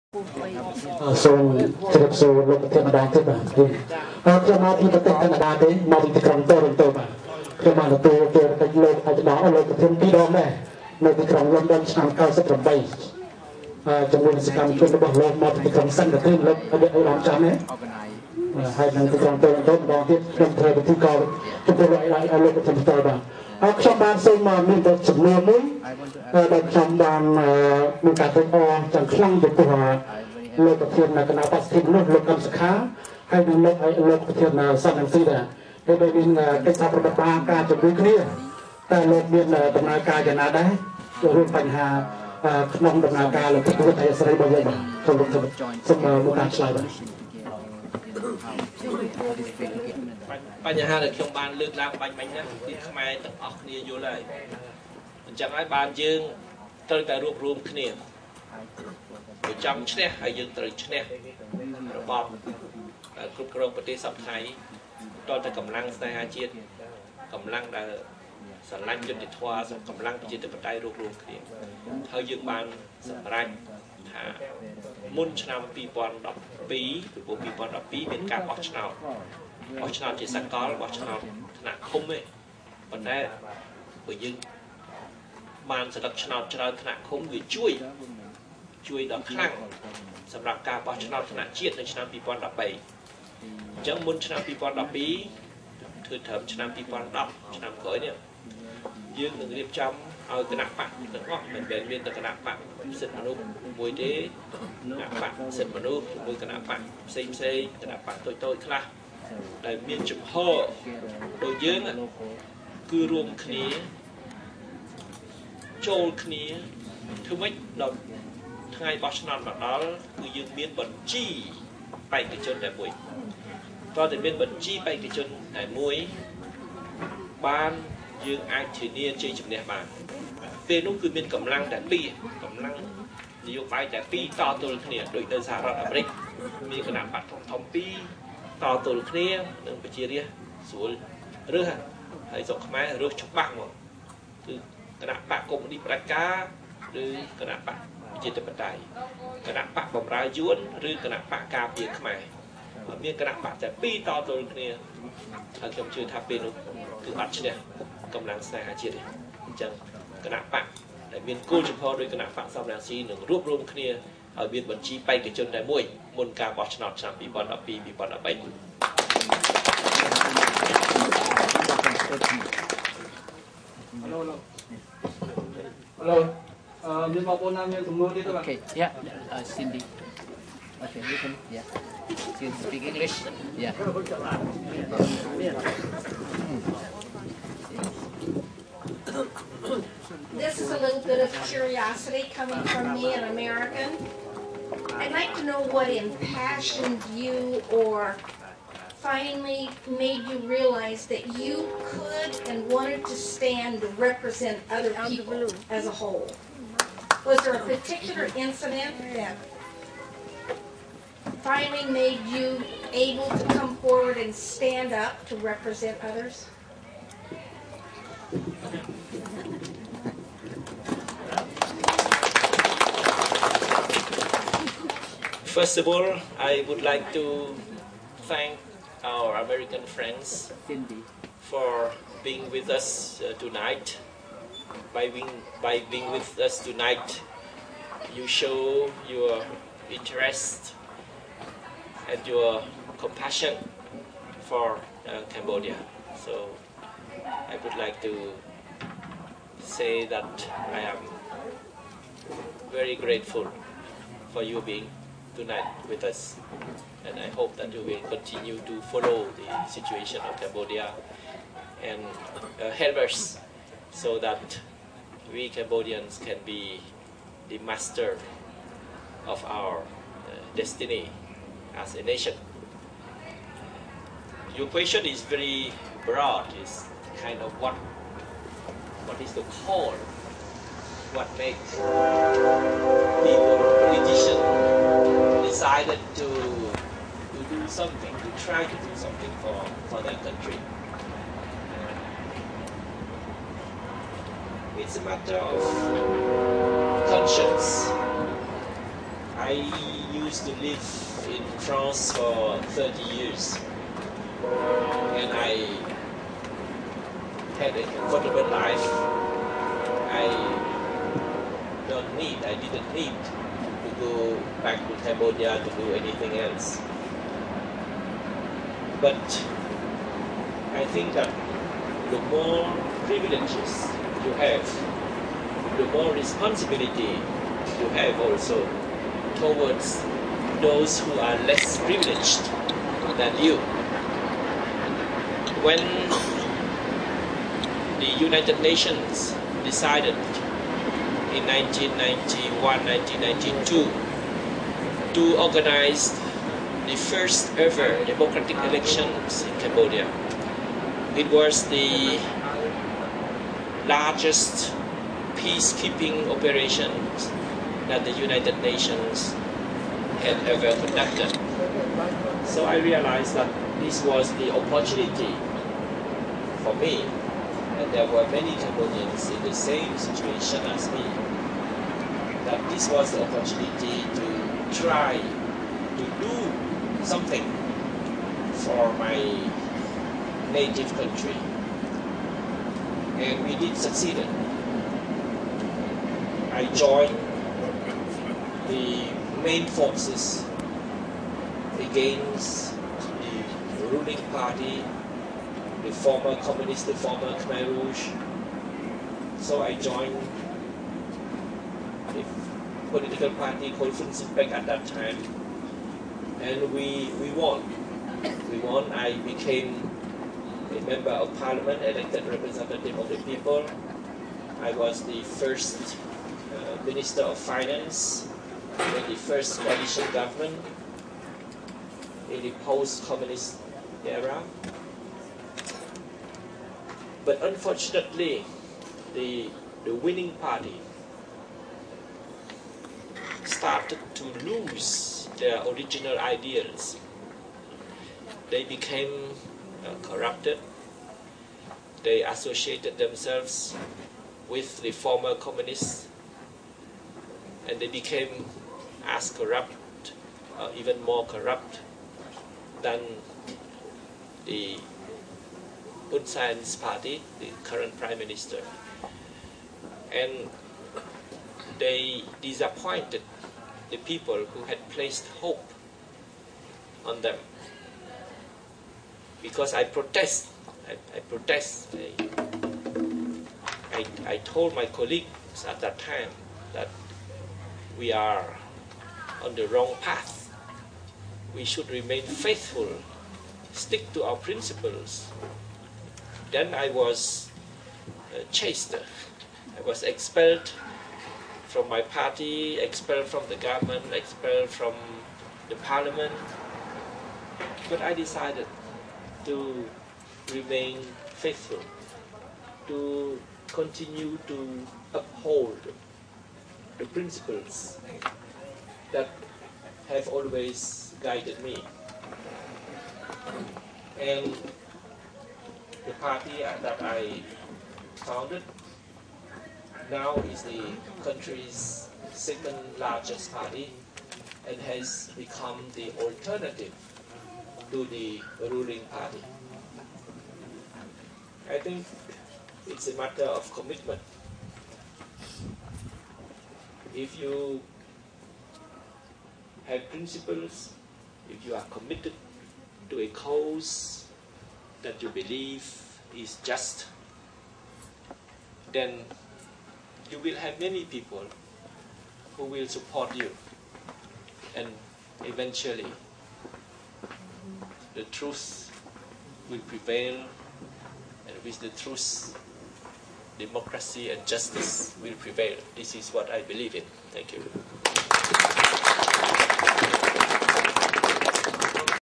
Mr. Sam Rainsy in Stockton, California - Part 7
Mr. Sam Rainsy delivered speech to members and supporters in Stockton, California on May 31, 2009.